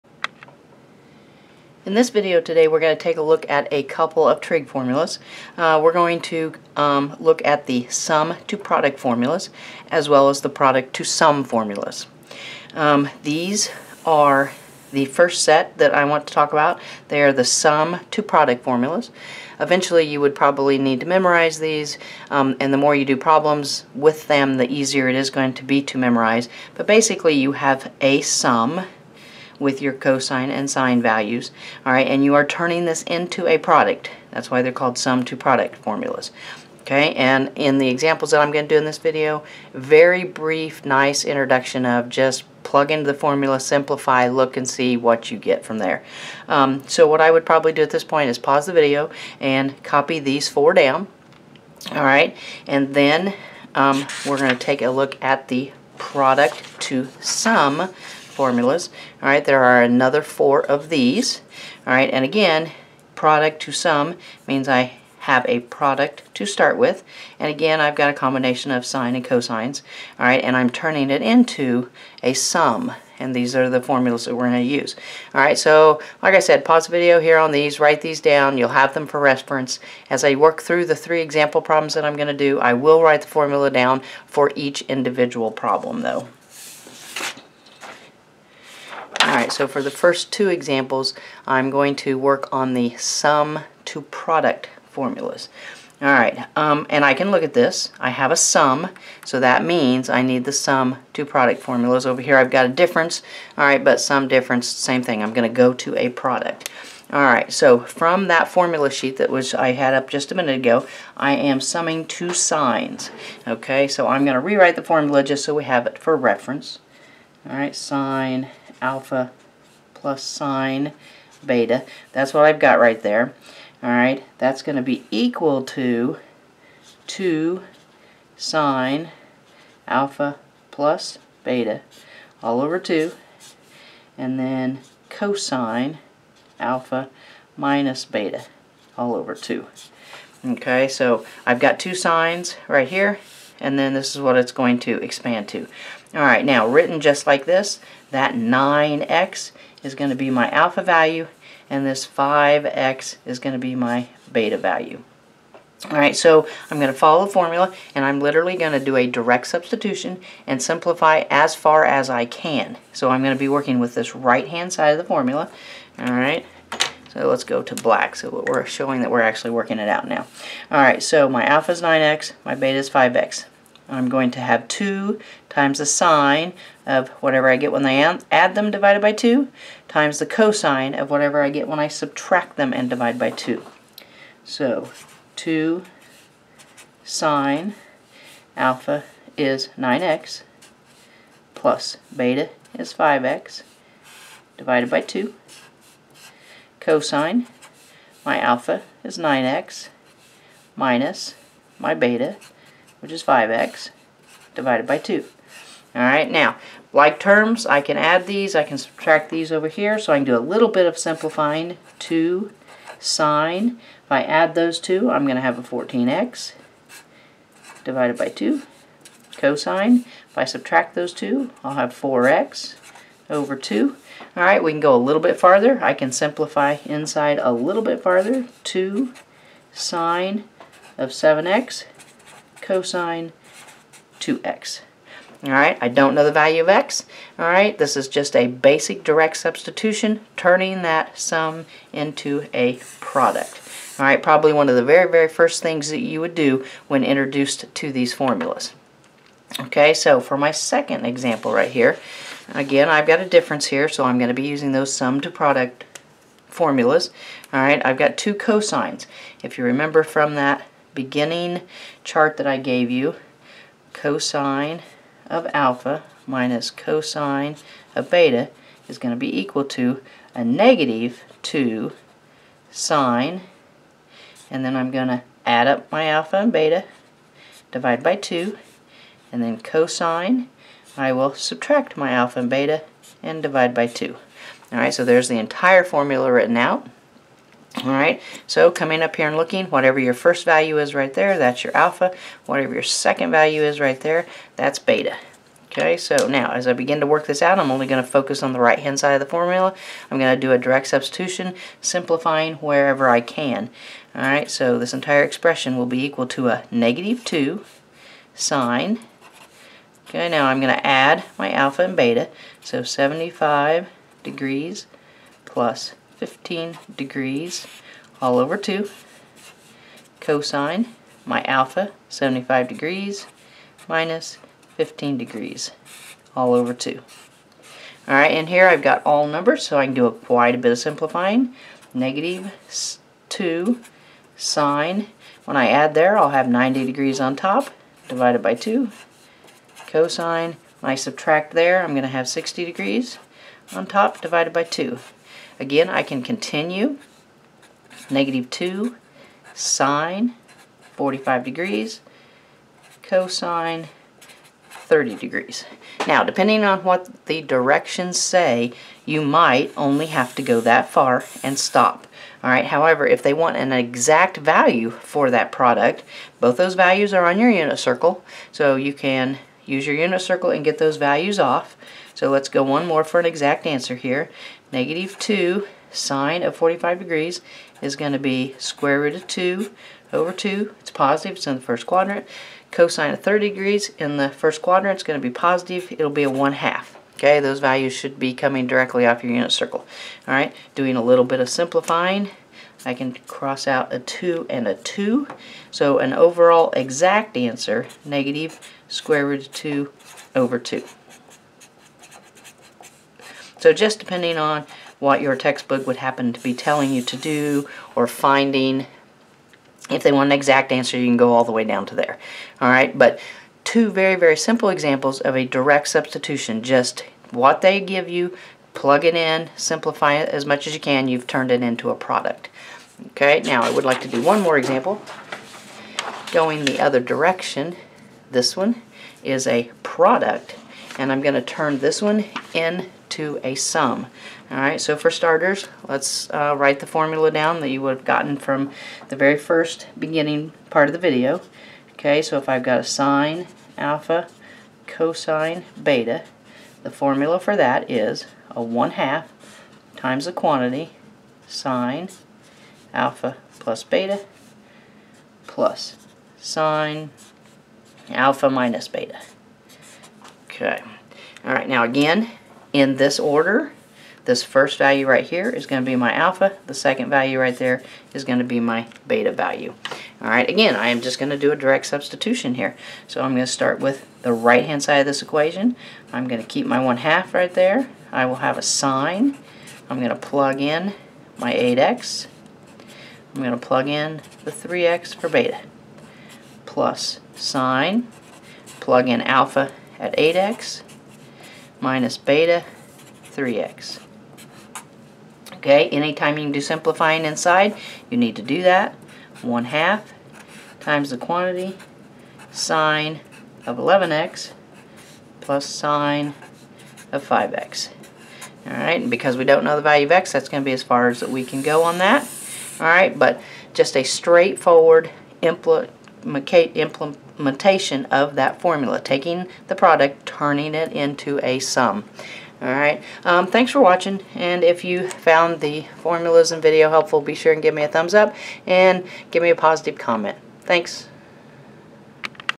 Trigonometry: Product to Sum & Sum to Product Formulas
This video goes through 3 examples using the Product to Sum Formulas and the Sum to Product Formulas.  The examples are all very basic examples showing a direct substitution into the formula and then simplifying.

MISTAKE:  The formula is written incorrectly in the second example.   However, the problem has been worked correctly, by using the correct values for the formula that should have been used.  IT SHOULD SAY SIN 30, WHICH IS 1/2